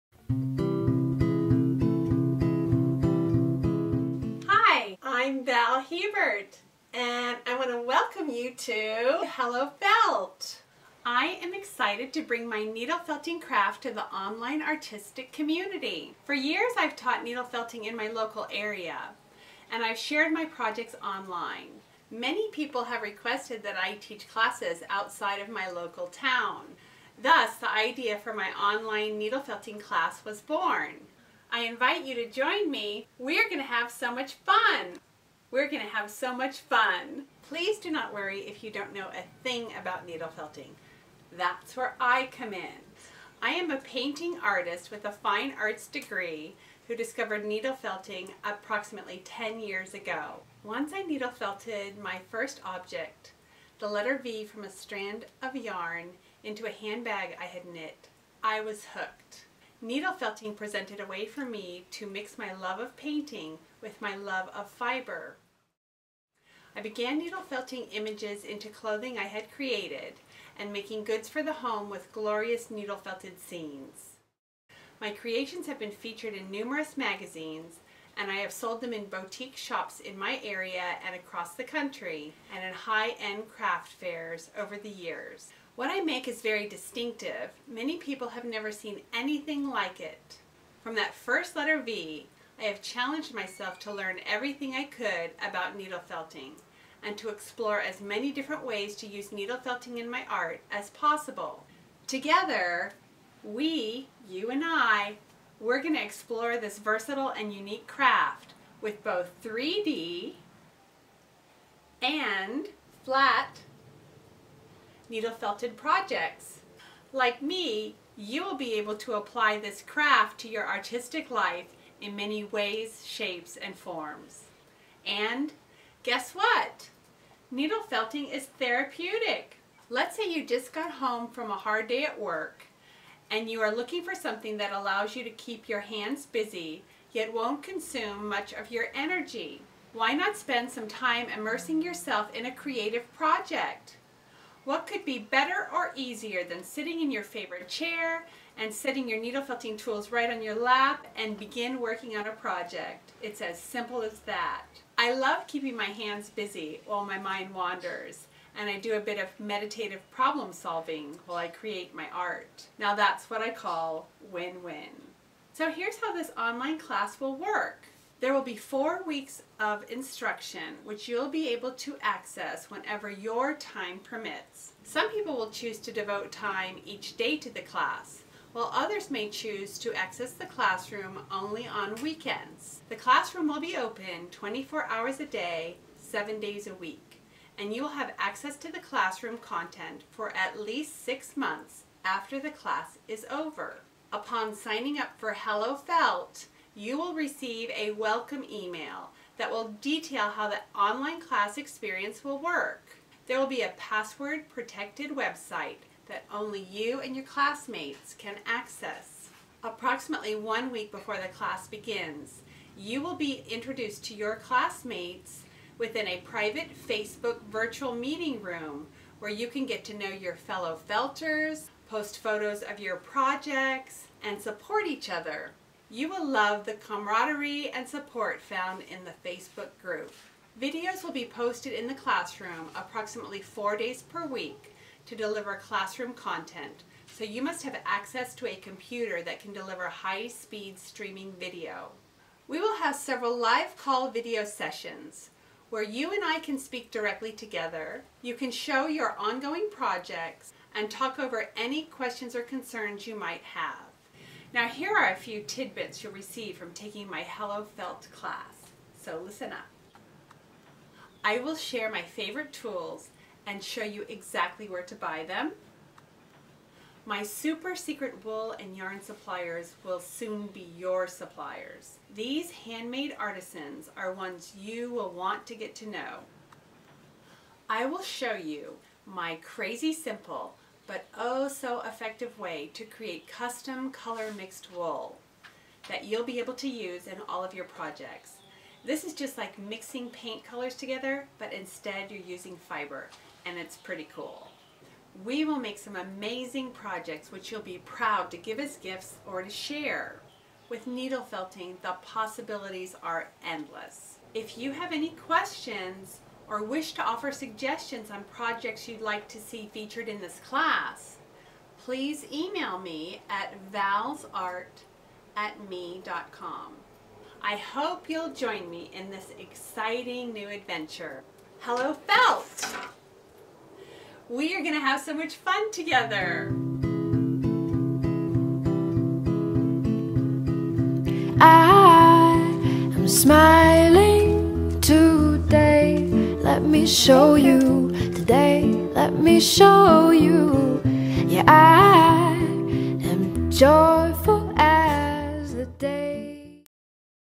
All about Hello Felt: the e-course
Introductory video to my upcoming e-course, Hello Felt! Learn about this upcoming 4 week e-course that will teach you all about needle felting. We will explore 3-D and flat needle felted projects, and have lots of fun in the process! Visit ValsArtStudio.com/hello-felt/ for more information!